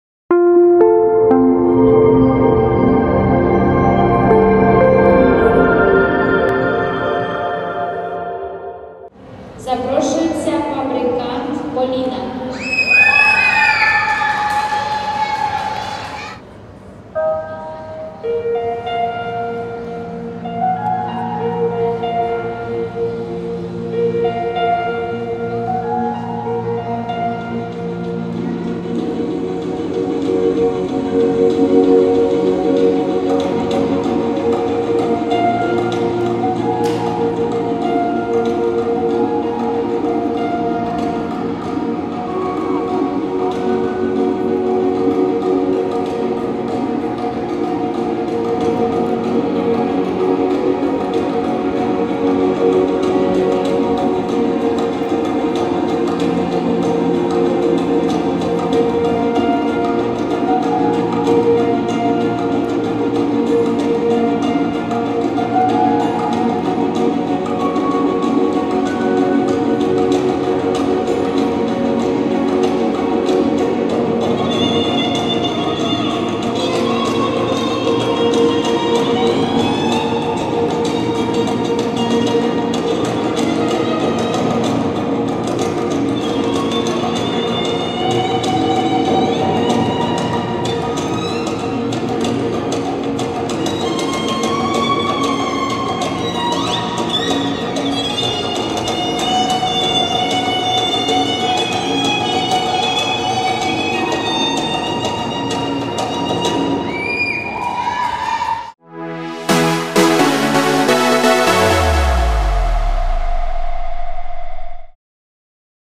Фабрикант Поліна 2013 обруч
VII Всеукраїнський   турнір з художньої гімнастики "ЛІТНЯ ФЕЄРІЯ 2020", м. Запоріжжя, СК, "Мотор-Січ", 18-19 серпня 2020